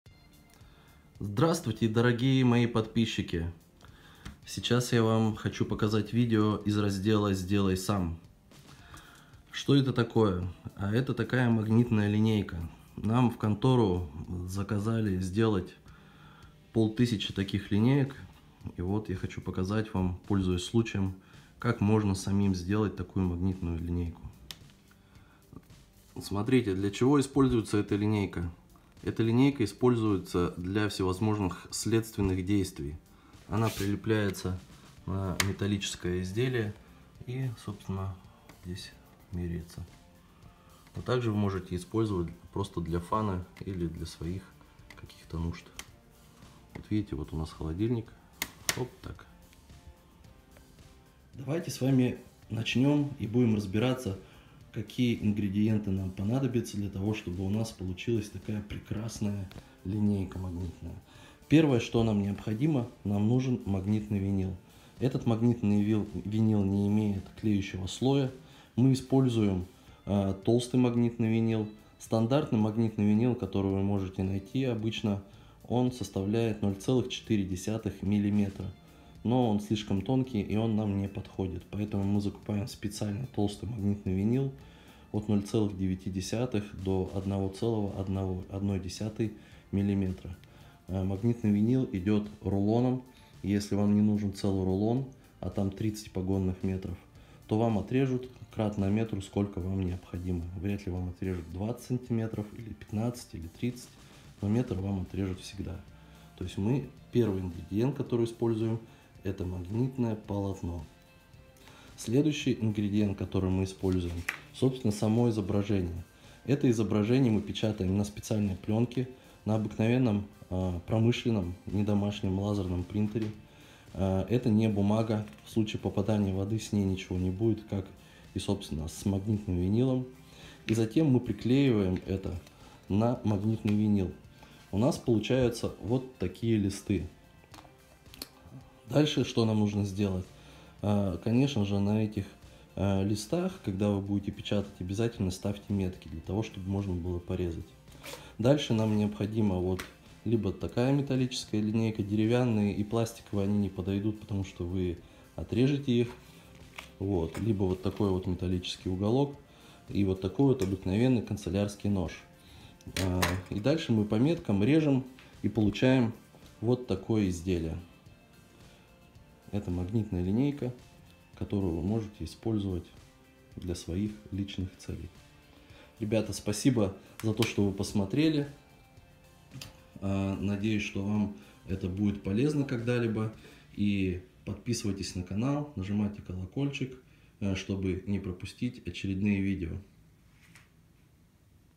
Как это сделано. Магнитная линейка. How it's made magnetic vinyl straightedge
Магнитный винил+руки из нормального места=отличный результат